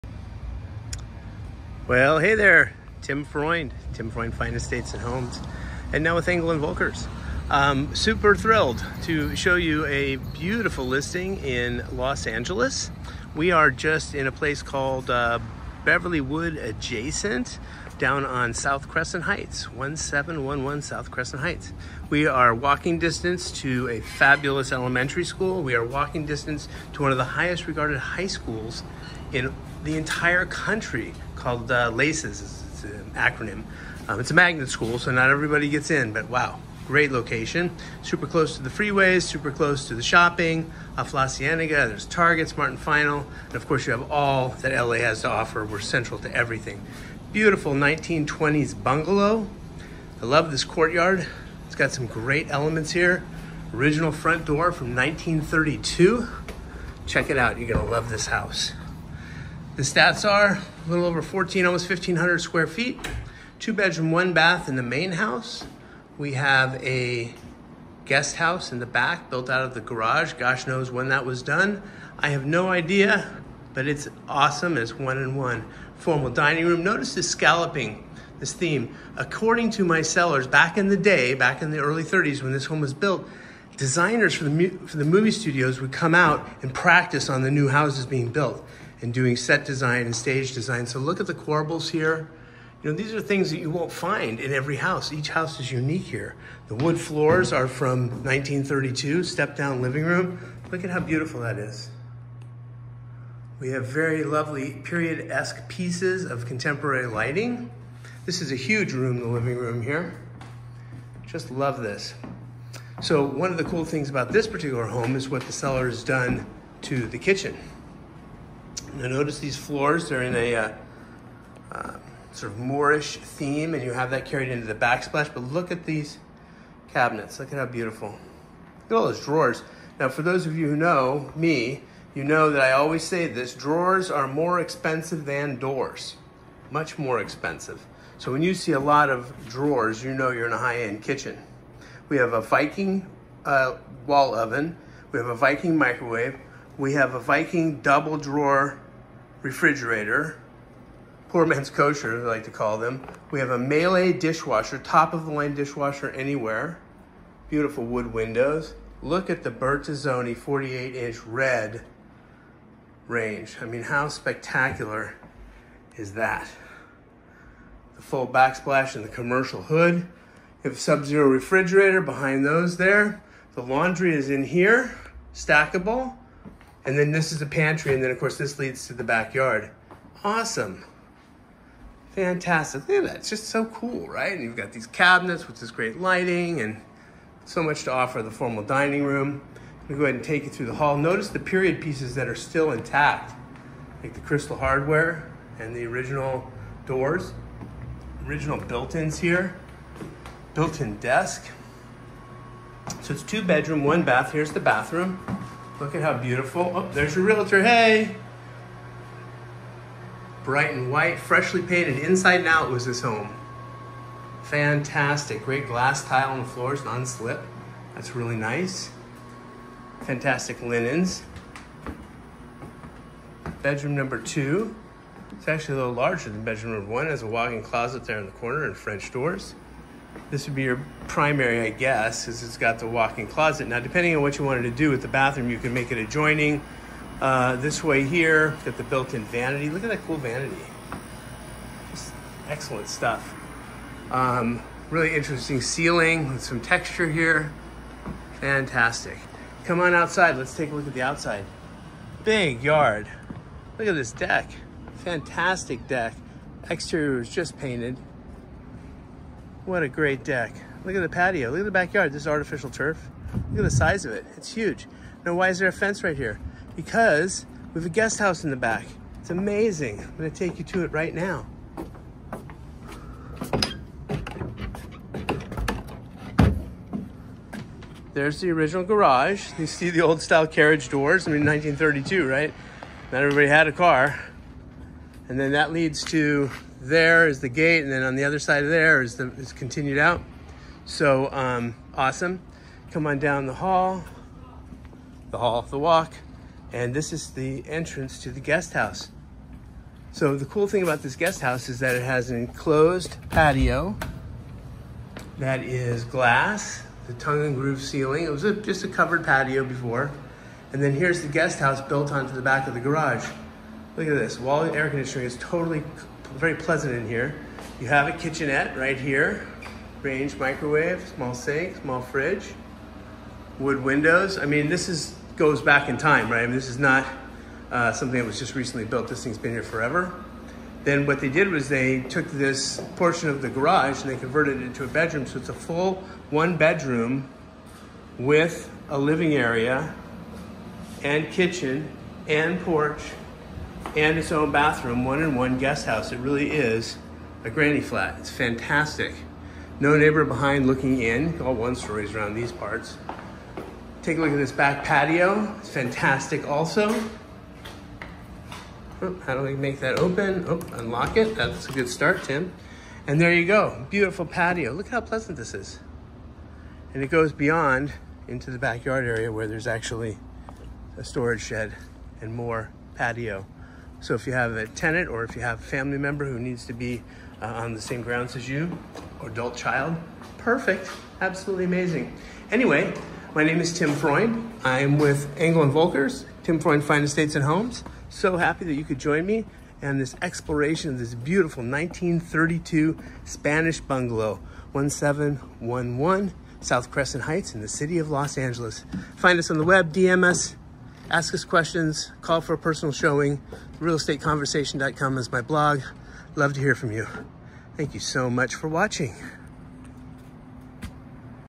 1711 S. Crescent Heights
Beverlywood adjacent. Close to everything!  Great shopping both big box and boutique. Freeway close and near wonderful schools!